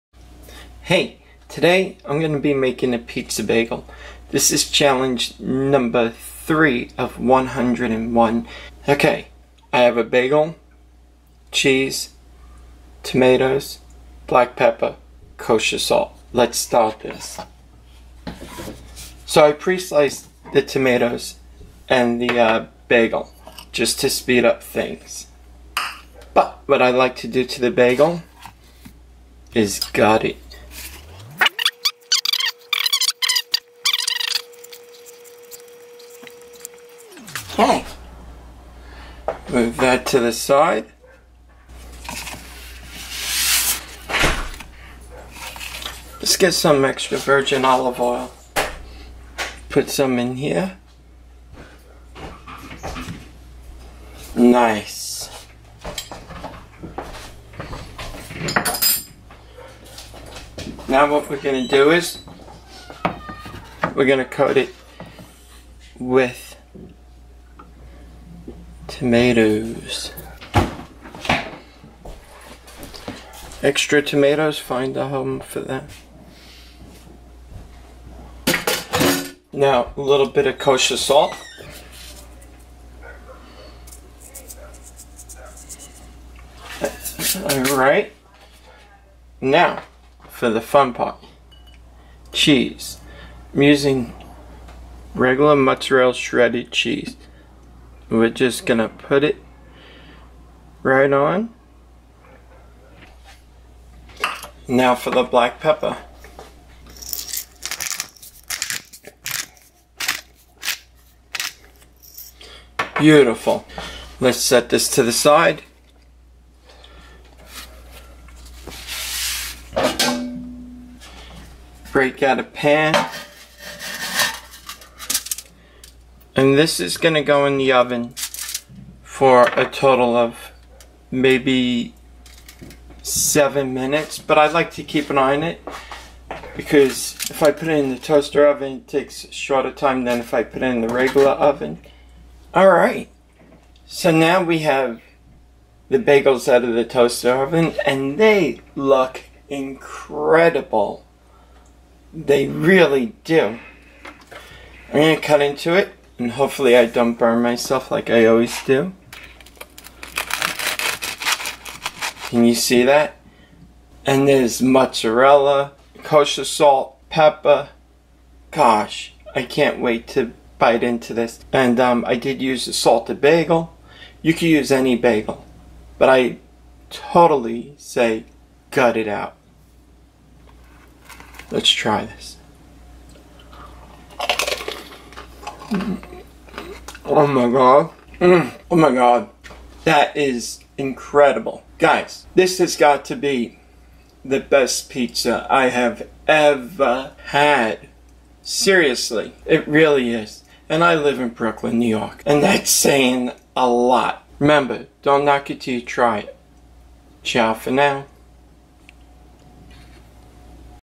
Pizza Bagel #3 of 101 Different Bagel Challenges
Being that i'm from Brooklyn, New York. my friends from around the world tell me how jealous that they are of me for have BAGEL, BREAD, & PIZZA at my finger tips.  - - - I don't know how good they are for i never leave a 3 mile radius.

1 Bagel it can be salted, garlic, onion, or everything
Must gut the bagel.
Slice up grape tomatoes or slice in half cherry tomatoes
If you want at this time you should add some kosher salt and basil (anything you like to spice it up).
Grab a cheese you love and put it on top, add toppings and pepper to taste.